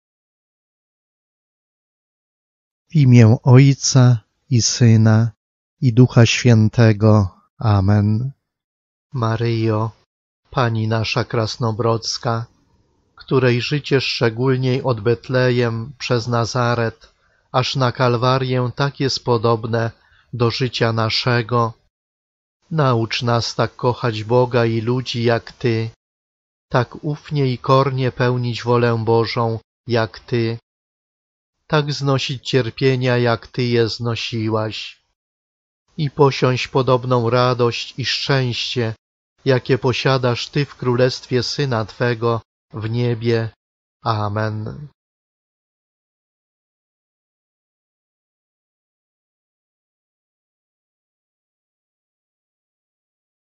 Modlitwa do Matki Bożej Krasnobrodzkiej o dobre życie
Modlitwa do Matki Boskiej Krasnobrodzkiej o dobre życie

  Maryjo, Pani nasza Krasnobrodzka, której życie szczególniej od Betlejem przez Nazaret aż na Kalwarię tak jest podobne do życia naszego naucz nas tak kochać Boga i ludzi jak Ty, tak ufnie i kornie pełnić wolę Bożą jak Ty, tak znosić cierpienia jak Ty je znosiłaś i posiąść podobną radość i szczęście jakie posiadasz Ty w Królestwie Syna Twego w niebie. Amen.